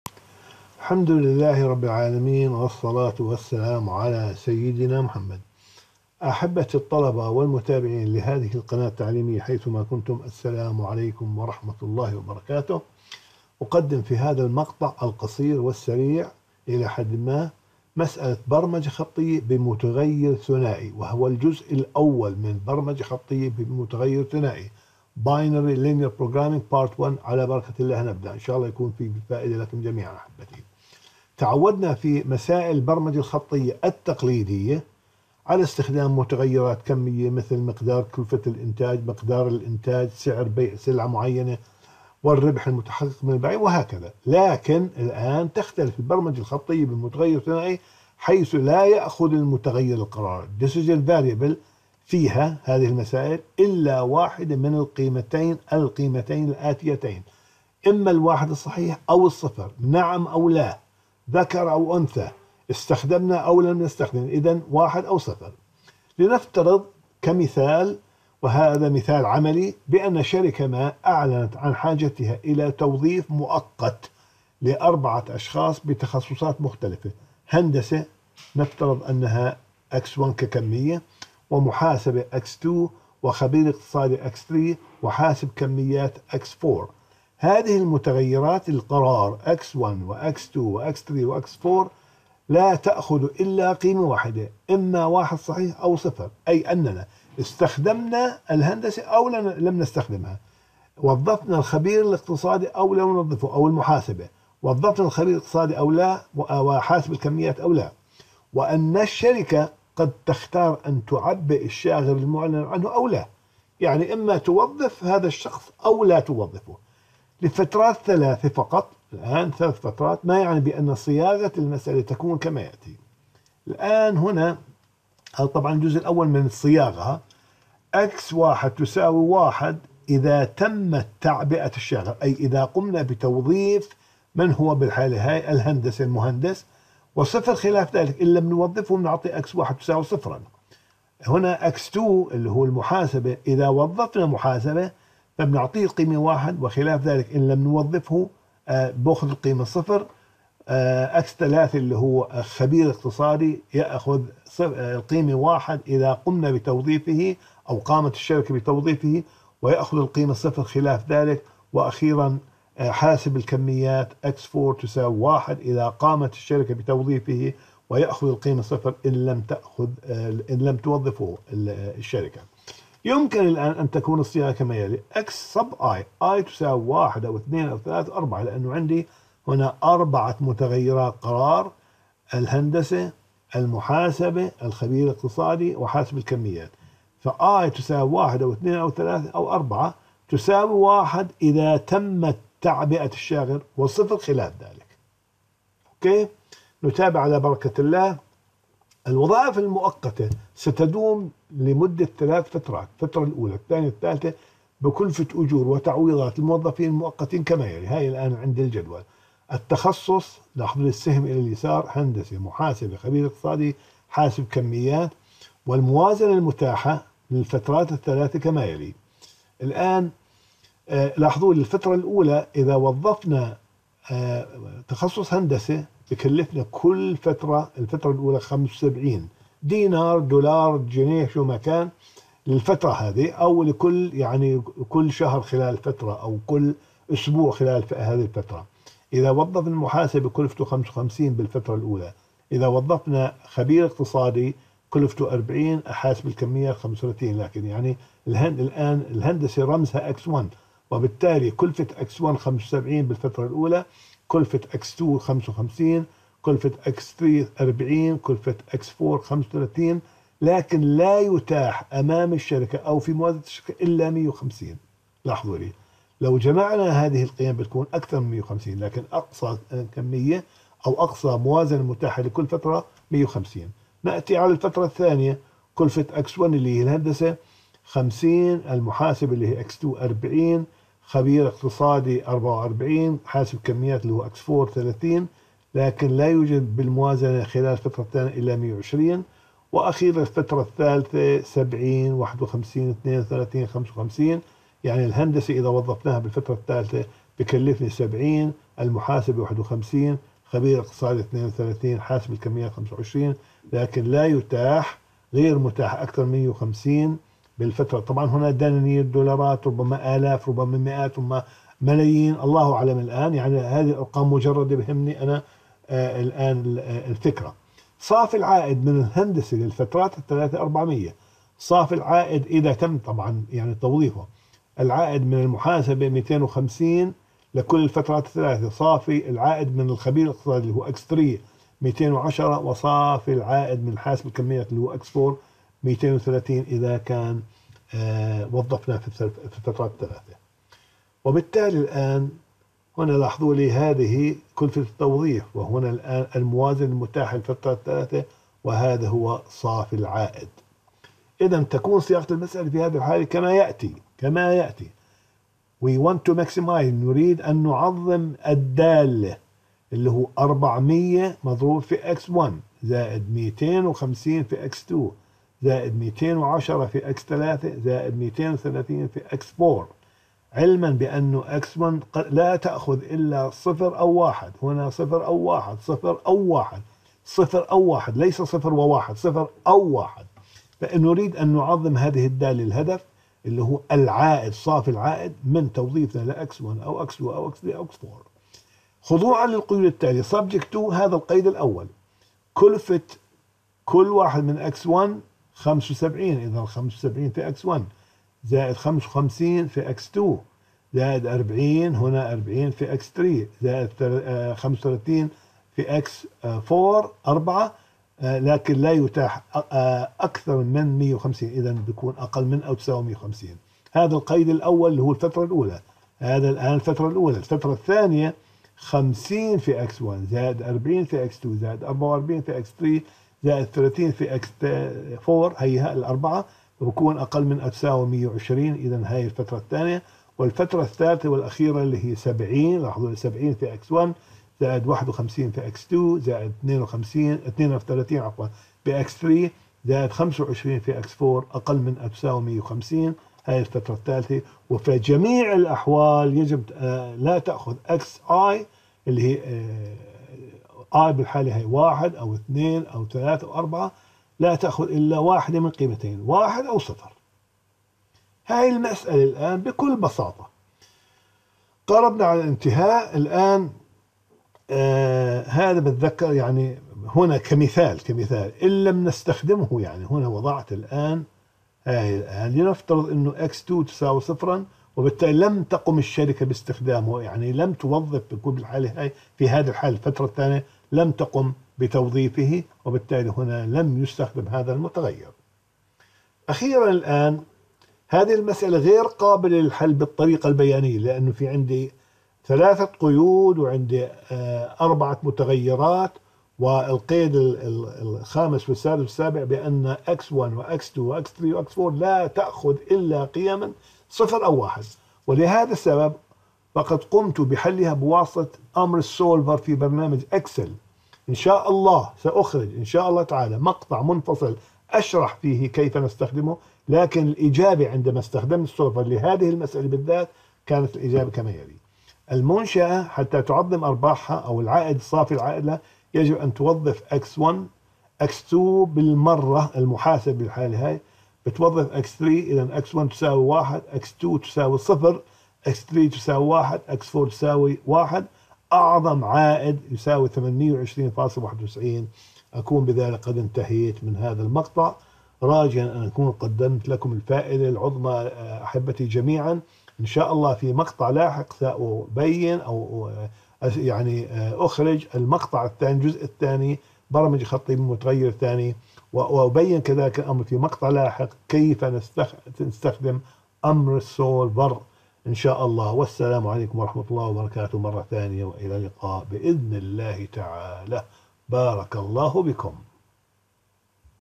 برمجة خطية (1) بمتغير ثنائي ... Binary - Integer LPP Part (1)
يشرح هذا المقطع الجزء (1) من برمجة خطية بمتغير ثنائي، مع مثال عملي بالأرقام ...  Binary Linear Programing Problem with a Numerical Example